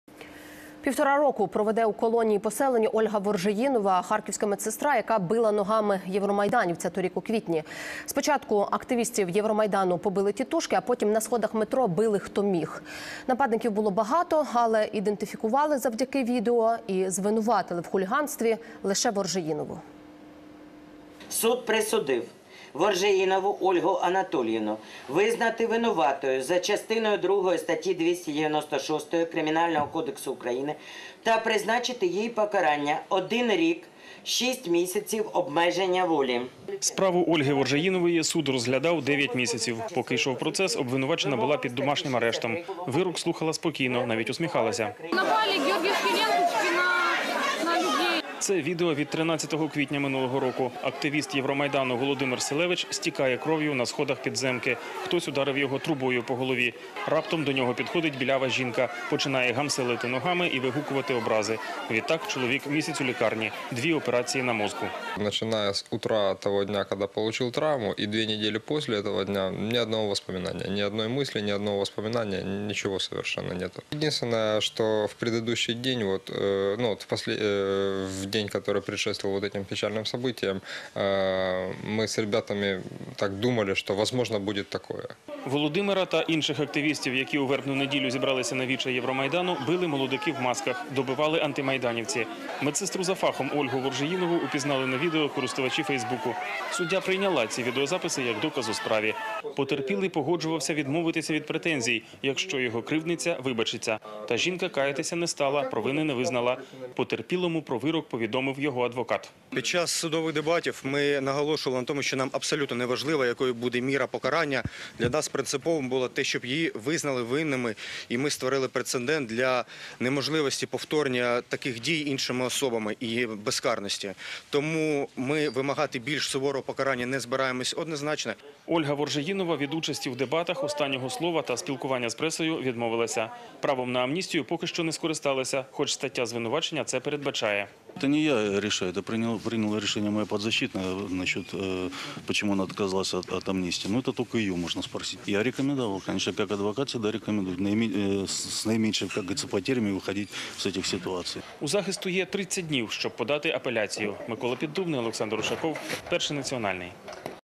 Жінку, яка била ногами євромайданівця, на півтора року позбавили волі
Півтора року проведе у колонії-поселенні Ольга Воржеїнова — харківська медсестра, яка била ногами євромайданівця торік у квітні. Спочатку активістів євромайдану побили тітушки, а потім на сходах метро били, хто міг. Нападників було багато, але ідентифікували завдяки відео і звинуватили в хуліганстві лише Воржеїнову.